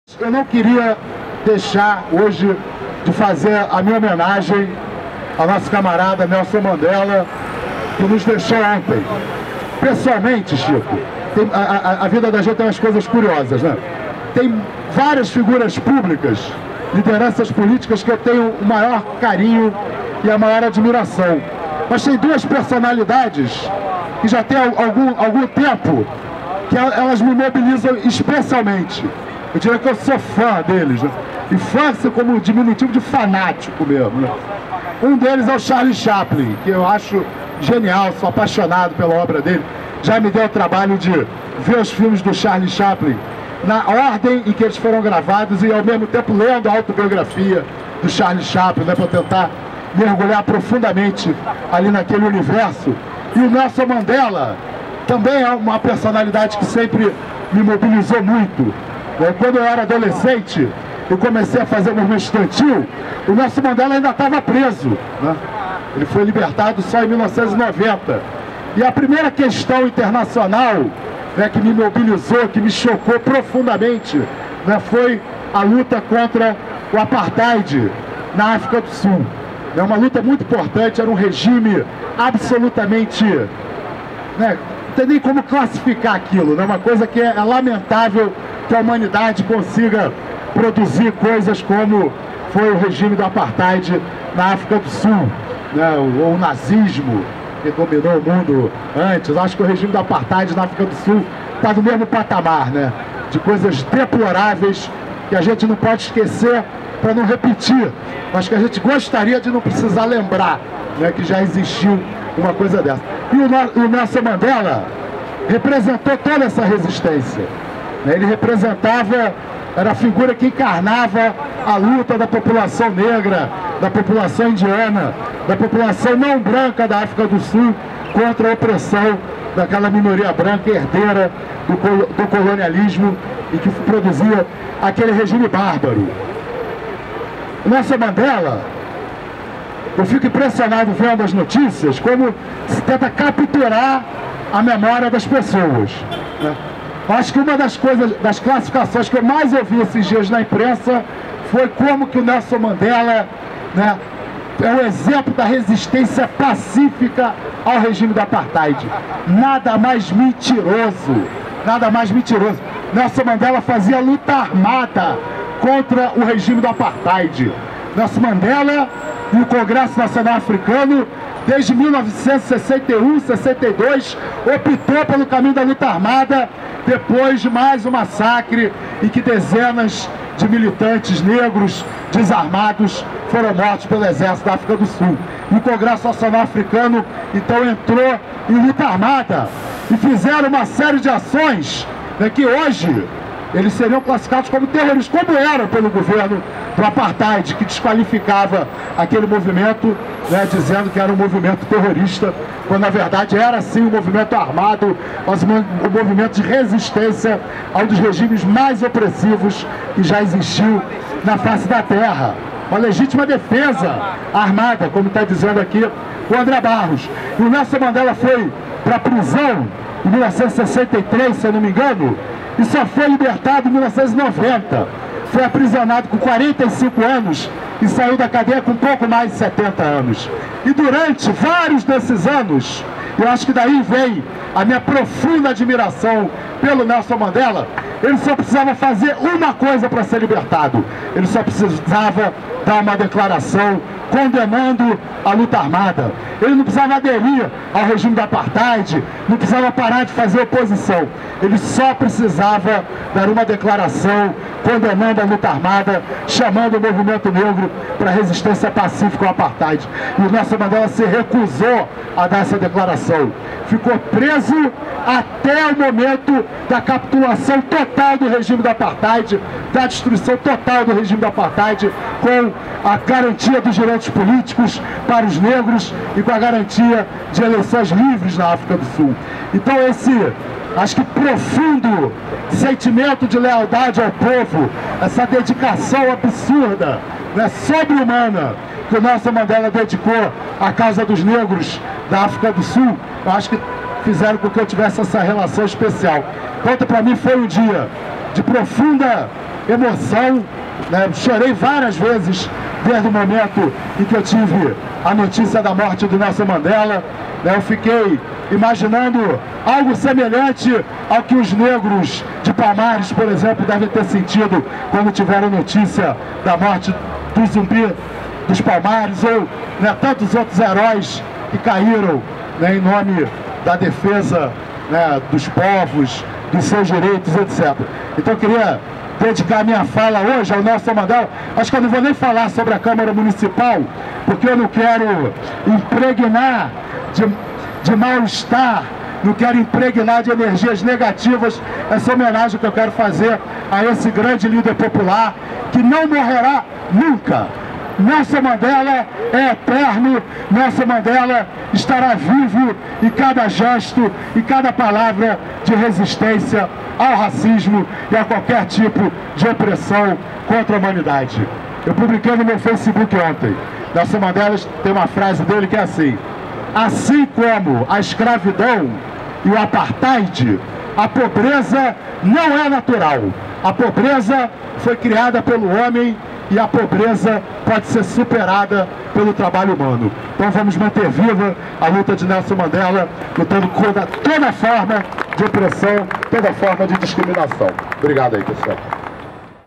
A história não contada de Nelson Mandela
Discuso do vereador Renato Cinco sobre a história política do ex-presidente Nelson Mandela. Em 06/12/2013.